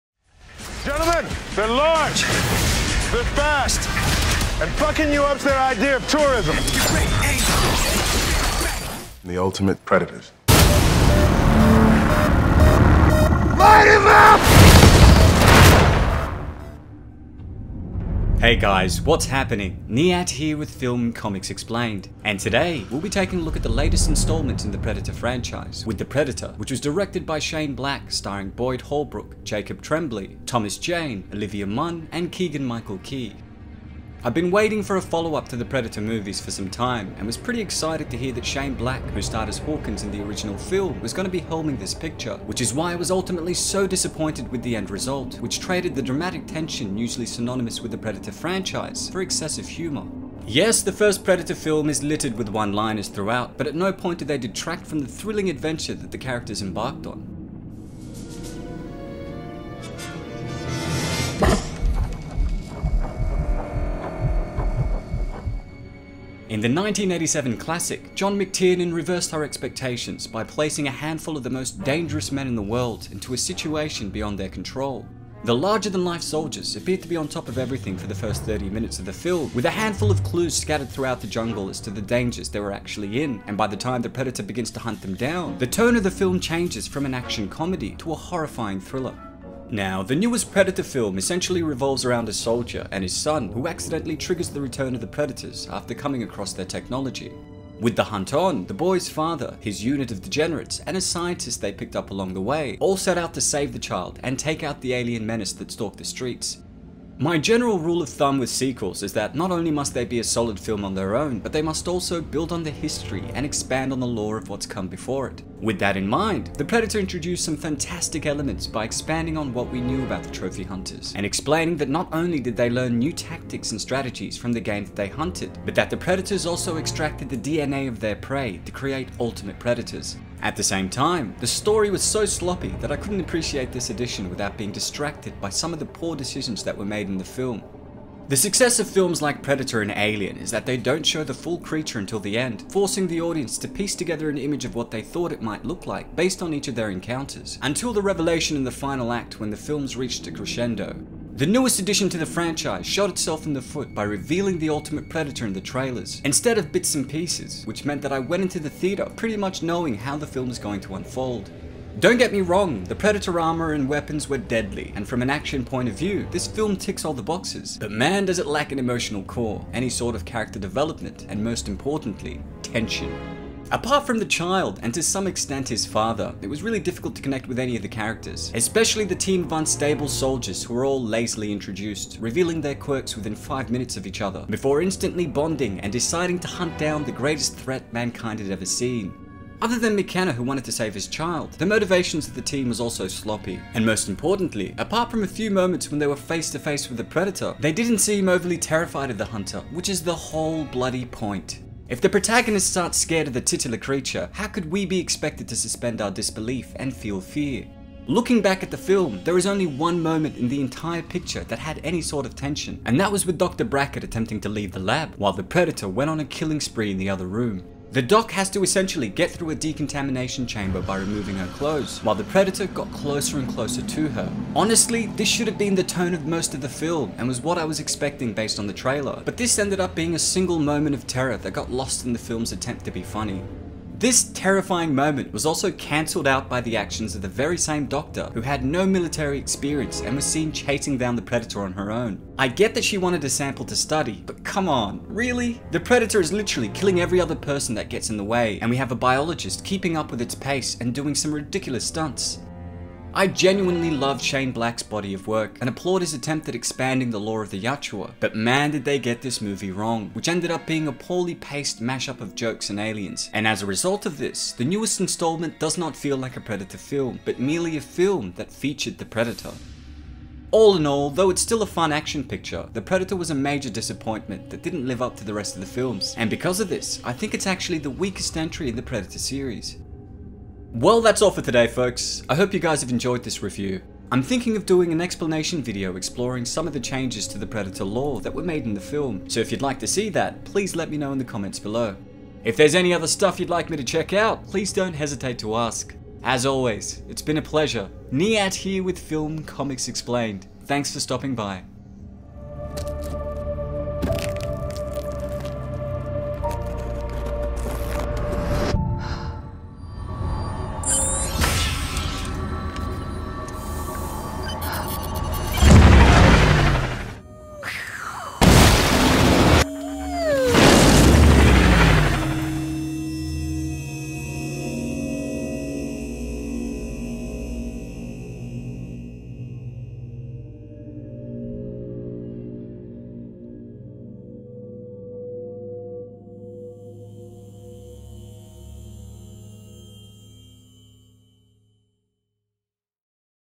The Predator (2018) Movie Review
The Predator is a 2018 American science fiction action comedy film directed by Shane Black and written by Black and Fred Dekker. It is the fourth installment in the Predator film series

ABOUT ME
This channel was created to assist in answering all of your questions about Films and Comics in an easy to understand way.